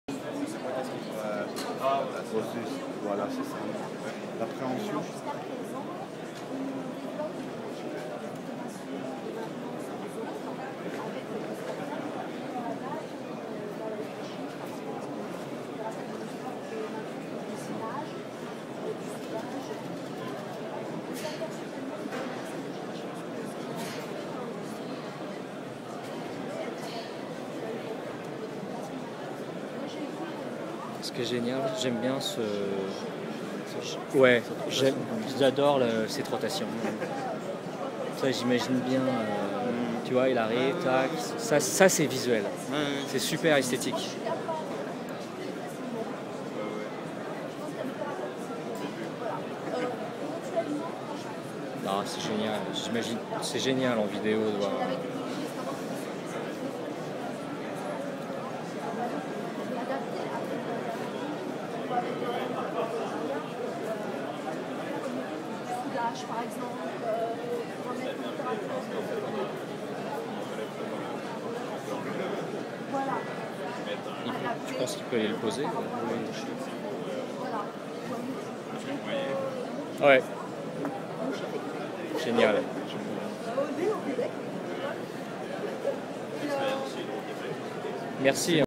Jaco, le bras robotisé de la société canadienne Kinova
Vidéo enregistrée par le 21 mars 2013 sur le salon Innorobo.